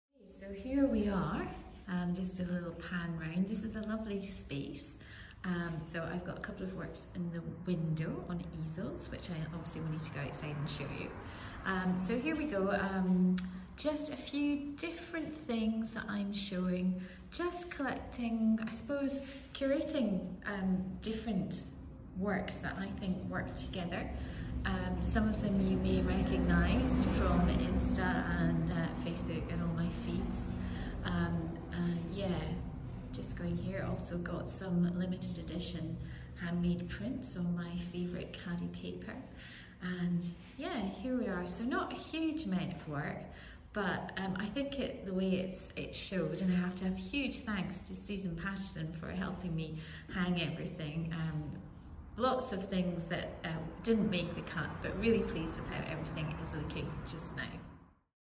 A walk round the exhibition
A quick tour of my first solo exhibition "Colour and Matter". The show was a great success with lots of people coming to see the work (and buy some of it). It was also a huge learning experience for me, but I think I will do it all again sometime.